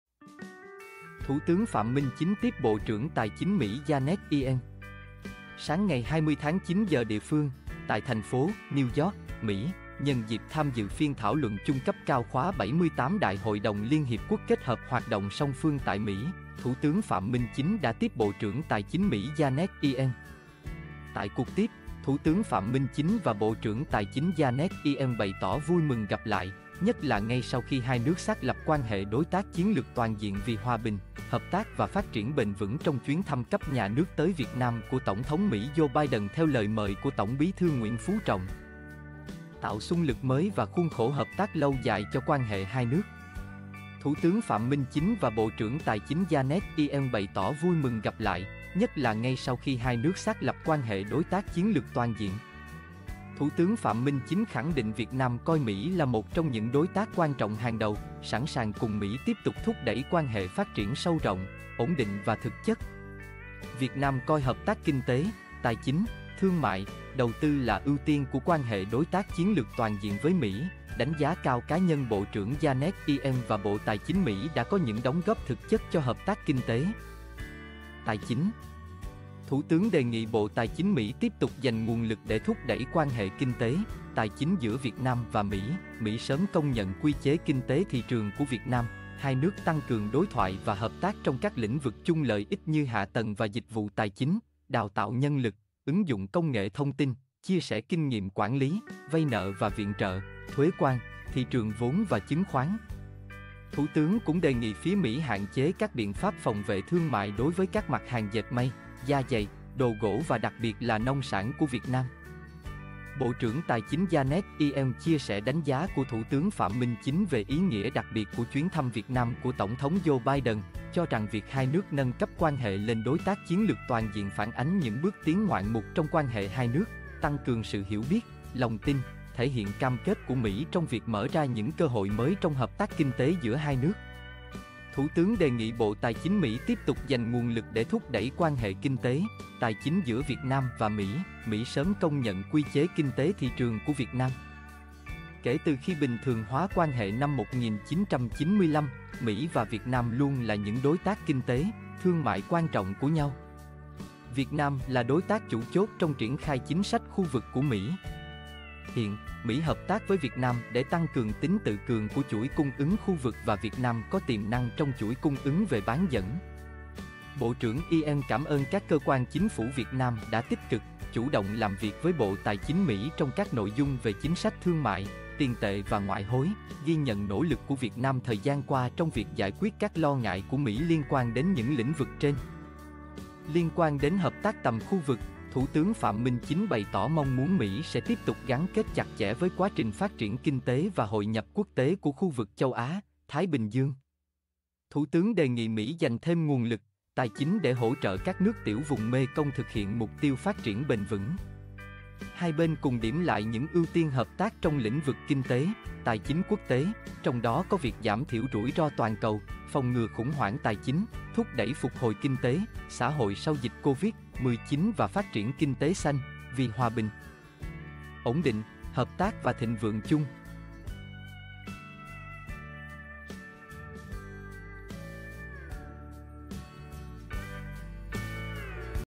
Thủ tướng Phạm Minh Chính tiếp Bộ trưởng Tài chính Mỹ Janet Yellen
Sáng 20.9 giờ địa phương, tại TP.New York (Mỹ), nhân dịp tham dự Phiên thảo luận chung cấp cao khóa 78 Đại hội đồng Liên Hiệp Quốc kết hợp hoạt động song phương tại Mỹ, Thủ tướng Phạm Minh Chính đã tiếp Bộ trưởng Tài chính Mỹ Janet Yellen.

Tại cuộc tiếp, Thủ tướng Phạm Minh Chính và Bộ trưởng Tài chính Janet Yellen bày tỏ vui mừng gặp lại, nhất là ngay sau khi hai nước xác lập quan hệ đối tác chiến lược toàn diện vì hòa bình, hợp tác và phát triển bền vững trong chuyến thăm cấp nhà nước tới Việt Nam của Tổng thống Mỹ Joe Biden theo lời mời của Tổng Bí thư Nguyễn Phú Trọng, tạo xung lực mới và khuôn khổ hợp tác lâu dài cho quan hệ hai nước.

Thủ tướng Phạm Minh Chính và Bộ trưởng Tài chính Janet Yellen bày tỏ vui mừng gặp lại, nhất là ngay sau khi hai nước xác lập quan hệ đối tác chiến lược toàn diện

Thủ tướng Phạm Minh Chính khẳng định Việt Nam coi Mỹ là một trong những đối tác quan trọng hàng đầu; sẵn sàng cùng Mỹ tiếp tục thúc đẩy quan hệ phát triển sâu rộng, ổn định và thực chất.

Việt Nam coi hợp tác kinh tế, tài chính, thương mại, đầu tư là ưu tiên của quan hệ đối tác chiến lược toàn diện với Mỹ; đánh giá cao cá nhân Bộ trưởng Janet Yellen và Bộ Tài chính Mỹ đã có những đóng góp thực chất cho hợp tác kinh tế, tài chính.

Thủ tướng đề nghị Bộ Tài chính Mỹ tiếp tục dành nguồn lực để thúc đẩy quan hệ kinh tế, tài chính giữa Việt Nam và Mỹ; Mỹ sớm công nhận Quy chế kinh tế thị trường của Việt Nam; hai nước tăng cường đối thoại và hợp tác trong các lĩnh vực chung lợi ích như hạ tầng và dịch vụ tài chính, đào tạo nhân lực, ứng dụng công nghệ thông tin, chia sẻ kinh nghiệm quản lý, vay nợ và viện trợ, thuế quan, thị trường vốn và chứng khoán.

Thủ tướng cũng đề nghị phía Mỹ hạn chế các biện pháp phòng vệ thương mại đối với các mặt hàng dệt may, da giày, đồ gỗ và đặc biệt là nông sản của Việt Nam.

Bộ trưởng Tài chính Janet Yellen chia sẻ đánh giá của Thủ tướng Phạm Minh Chính về ý nghĩa đặc biệt của chuyến thăm Việt Nam của Tổng thống Joe Biden, cho rằng việc hai nước nâng cấp quan hệ lên đối tác chiến lược toàn diện phản ánh những bước tiến ngoạn mục trong quan hệ hai nước, tăng cường sự hiểu biết, lòng tin, thể hiện cam kết của Mỹ trong việc mở ra những cơ hội mới trong hợp tác kinh tế giữa hai nước.

Thủ tướng đề nghị Bộ Tài chính Mỹ tiếp tục dành nguồn lực để thúc đẩy quan hệ kinh tế, tài chính giữa Việt Nam và Mỹ; Mỹ sớm công nhận Quy chế kinh tế thị trường của Việt Nam

Kể từ khi bình thường hóa quan hệ năm 1995, Mỹ và Việt Nam luôn là những đối tác kinh tế, thương mại quan trọng của nhau. Việt Nam là đối tác chủ chốt trong triển khai chính sách khu vực của Mỹ. Hiện, Mỹ hợp tác với Việt Nam để tăng cường tính tự cường của chuỗi cung ứng khu vực và Việt Nam có tiềm năng trong chuỗi cung ứng về bán dẫn.

Bộ trưởng Yellen cảm ơn các cơ quan Chính phủ Việt Nam đã tích cực, chủ động làm việc với Bộ Tài chính Mỹ trong các nội dung về chính sách thương mại, tiền tệ và ngoại hối; ghi nhận nỗ lực của Việt Nam thời gian qua trong việc giải quyết các lo ngại của Mỹ liên quan đến những lĩnh vực trên.

Liên quan đến hợp tác tầm khu vực, Thủ tướng Phạm Minh Chính bày tỏ mong muốn Mỹ sẽ tiếp tục gắn kết chặt chẽ với quá trình phát triển kinh tế và hội nhập quốc tế của khu vực châu Á - Thái Bình Dương. Thủ tướng đề nghị Mỹ dành thêm nguồn lực, tài chính để hỗ trợ các nước tiểu vùng Mê Kông thực hiện mục tiêu phát triển bền vững.

Hai bên cùng điểm lại những ưu tiên hợp tác trong lĩnh vực kinh tế, tài chính quốc tế, trong đó có việc giảm thiểu rủi ro toàn cầu, phòng ngừa khủng hoảng tài chính, thúc đẩy phục hồi kinh tế - xã hội sau dịch Covid-19 và phát triển kinh tế xanh, vì hòa bình, ổn định, hợp tác và thịnh vượng chung.